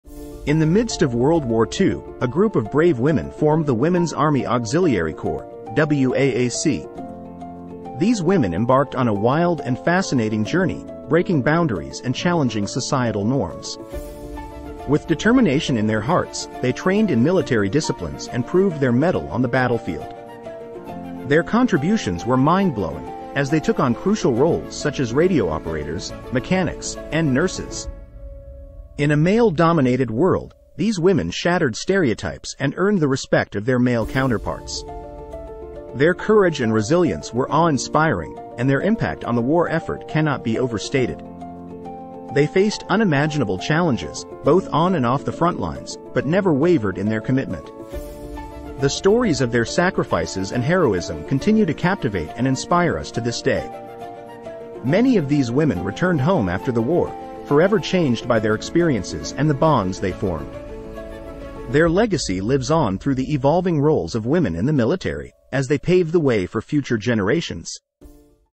Women's Army Auxiliary Corps was established in U.S. , allowing women to serve in non-combat.
Random facts!